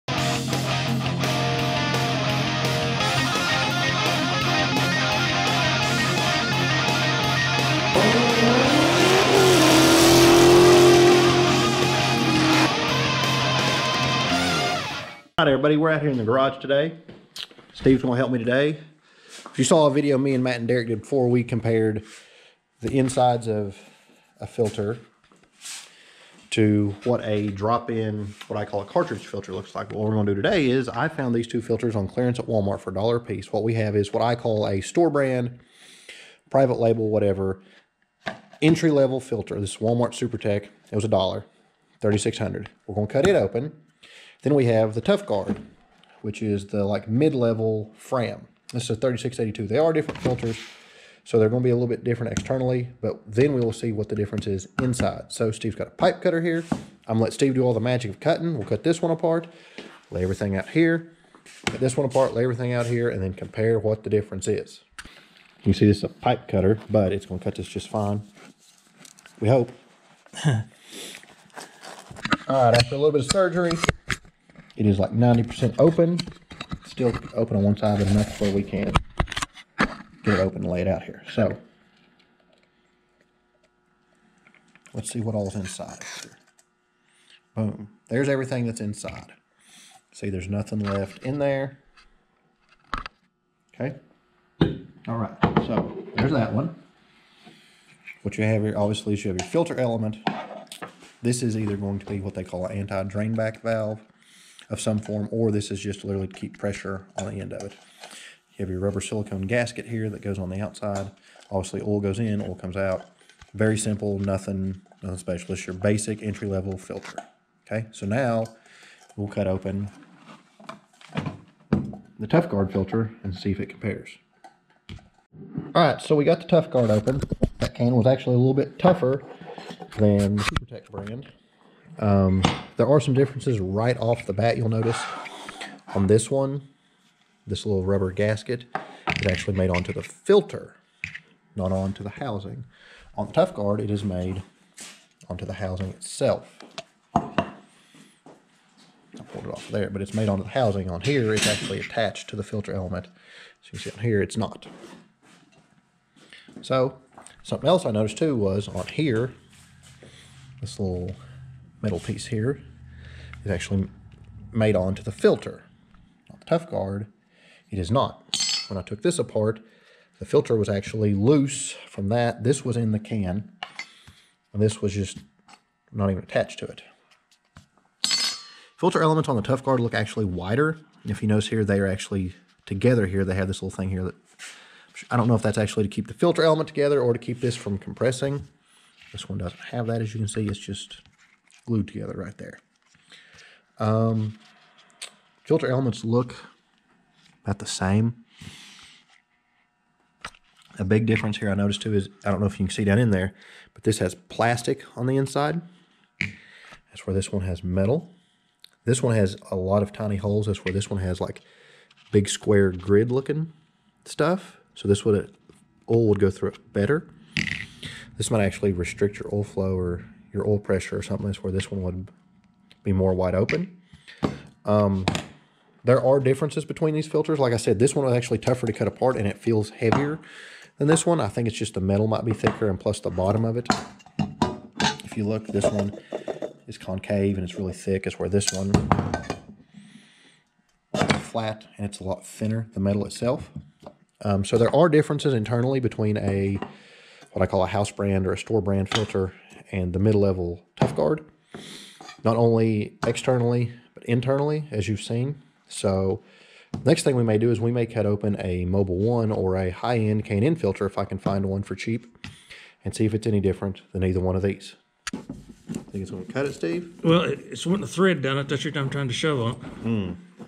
Supertech vs Fram Tough Guard oil filter : What's really inside?
For as long as I"ve been doing my own oil, I've always been curious what's the difference internally between the different cost levels of oil filters. I found some on clearance "down to the Wal-Marts" and picked them up. Now, let's do some surgery and find out what's inside them!